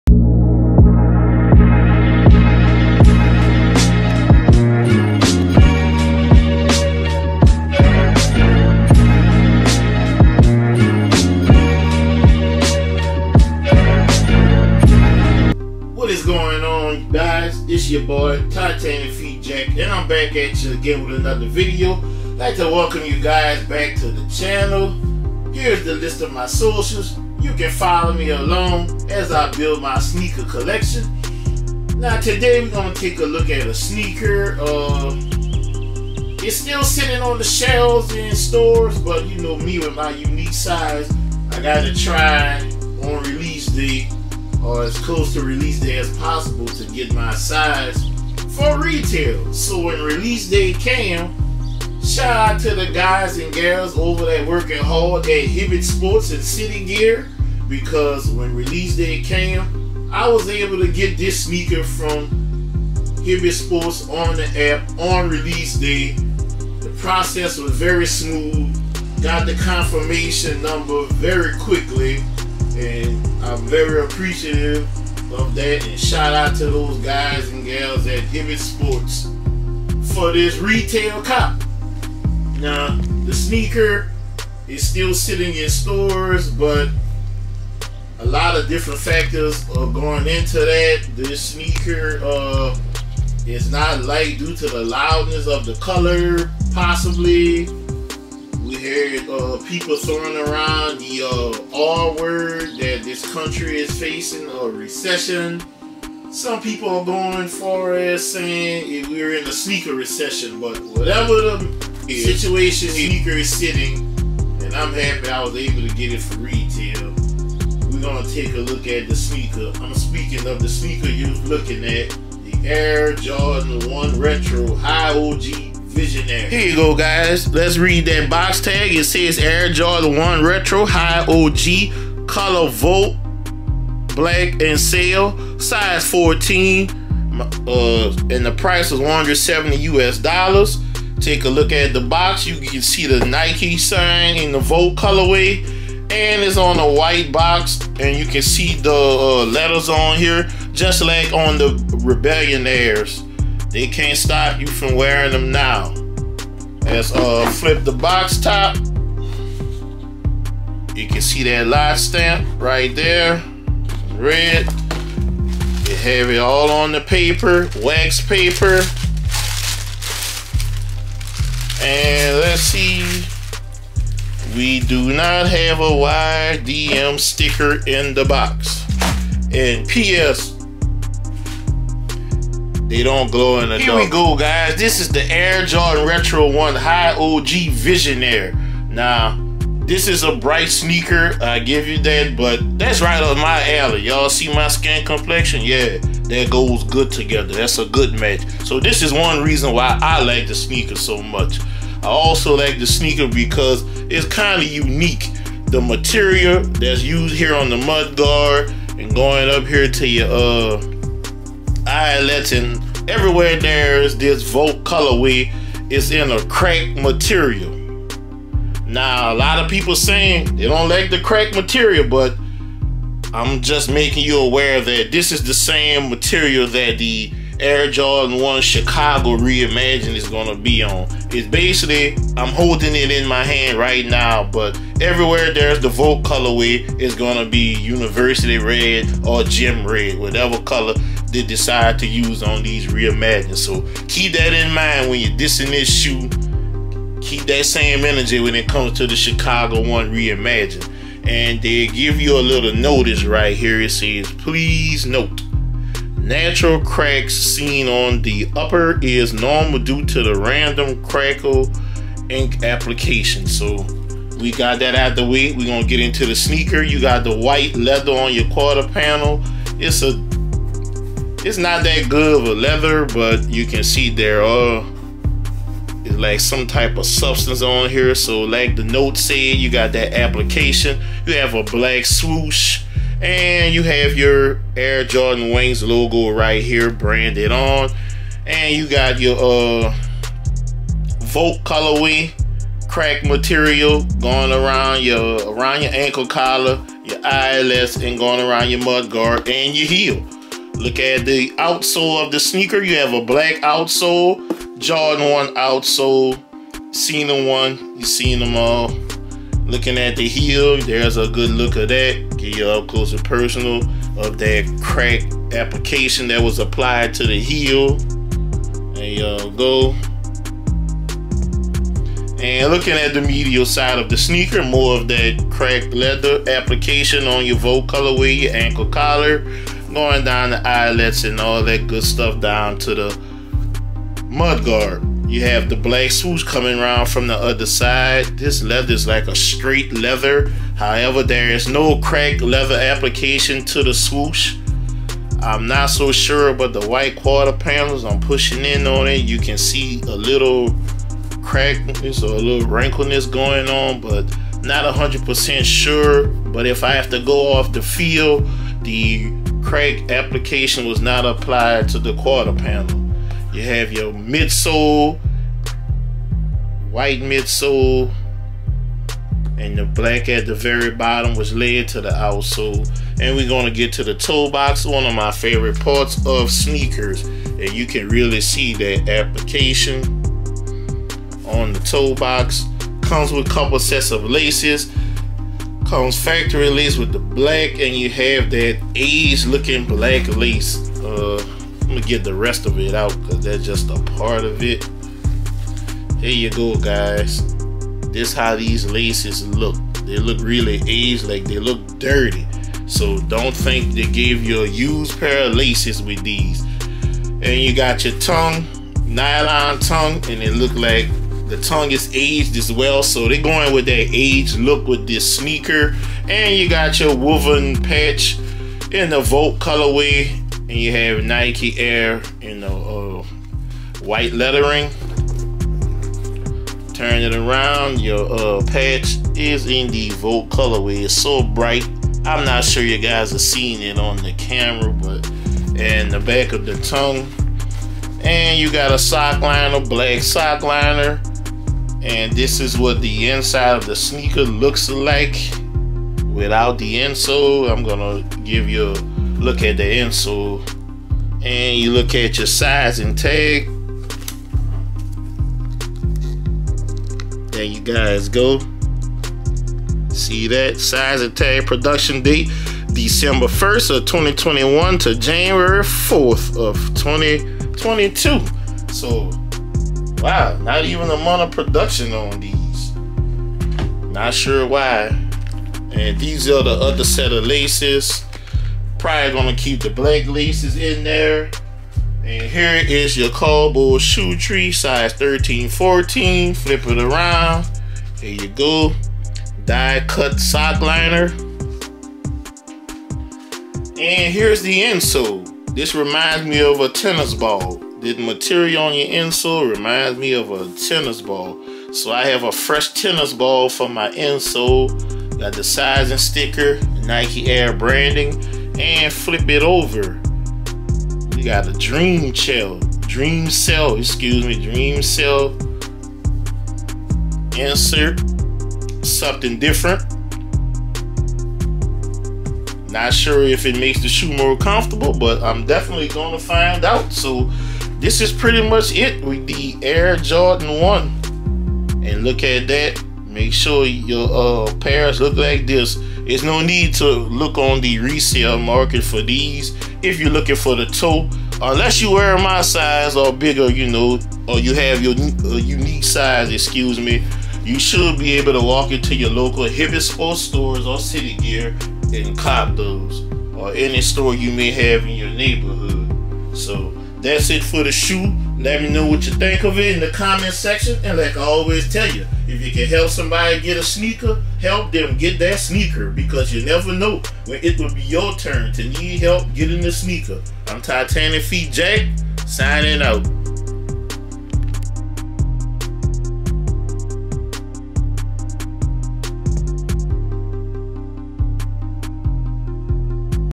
AIR JORDAN 1 HIGH OG “VISIONAIRE”
TitanicFeet Jack
190 Belle Terre Blvd. no. 2753

Size 15 in  Foamposites, Air Max, Jordan Retro 3, Jordan Retro 11, Yeezy 380, Kobe's

The Air Jordan 1 Retro High OG “Visionaire” delivers a fluorescent colorway of the legendary sneaker that started it all. The high top dons an upper that combines a smooth white leather base with cracked leather overlays in eye catching volt. Contrasting black accents land on the signature Swoosh, cracked leather collar, and the retro wings logo imprinted on the lateral ankle. A final hit of volt appears on the Nike Air tag that decorates the breathable nylon tongue. Lightweight cushioning is provided by an Air sole unit encapsulated in the heel of the rubber cupsole.